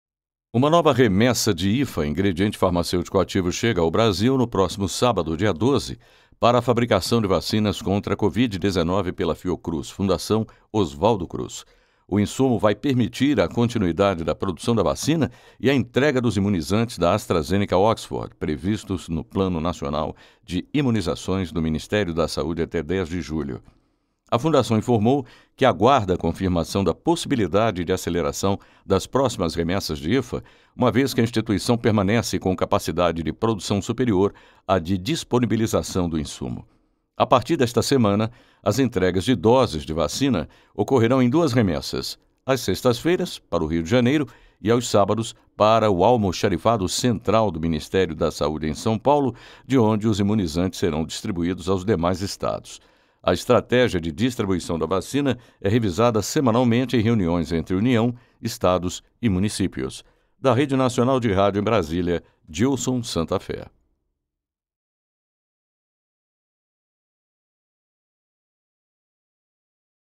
08-06-2021 - FIOCRUZ RECEBE NOVA REMESSA DE IFA NO PRÓXIMO SÁBADO
Insumo vai permitir a continuidade da produção da vacina e a entrega dos imunizantes da Astrazeneca/Oxford ao Plano Nacional de Imunizações, do Ministério da Saúde, até 10 de julho.

Uma nova remessa de IFA, Ingrediente Farmacêutico Ativo, chega ao Brasil no próximo sábado (12) para a fabricação de vacinas contra a Covid-19 pela Fiocruz, Fundação Oswaldo Cruz.

O insumo vai permitir a continuidade da produção da vacina e a entrega dos imunizantes da Astrazeneca/Oxford previstos no Plano Nacional de Imunizações, do Ministério da Saúde, até 10 de julho.

A fundação informou que "aguarda a confirmação da possibilidade de aceleração das próximas remessas de IFA, uma vez que a instituição permanece com capacidade de produção superior a de disponibilização do insumo".

A partir desta semana, as entregas de doses de vacina ocorrerão em duas remessas: às sextas-feiras, para o Rio de Janeiro; e aos sábados, para o almoxarifado central do Ministério da Saúde, em São Paulo, de onde os imunizantes serão distribuídos aos demais estados.

A estratégia de distribuição da vacina é revisada semanalmente em reuniões entre União, estados e municípios.